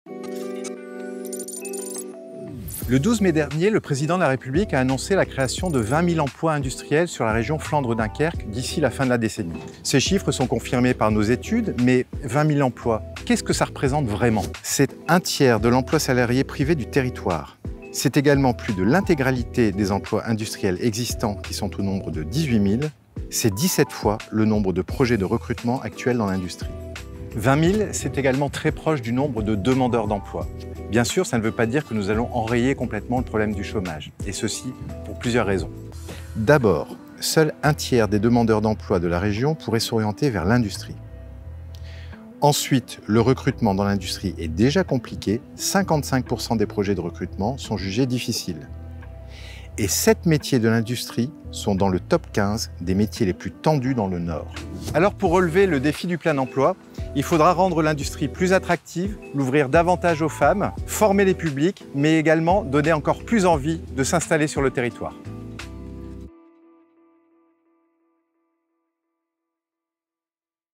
VIGILAB - 20 000 emplois dans le Dunkerquois
Le Président de la République a annoncé la création de 20.000 emplois industriels dans le territoire Flandre-Dunkerque avant la fin de la décennie 2020. Mais 20.000 emplois, qu’est-ce que cela représente concrètement ? La réponse en une 1 minute dans ce premier épisode vidéo du VIGILAB de l’AGUR.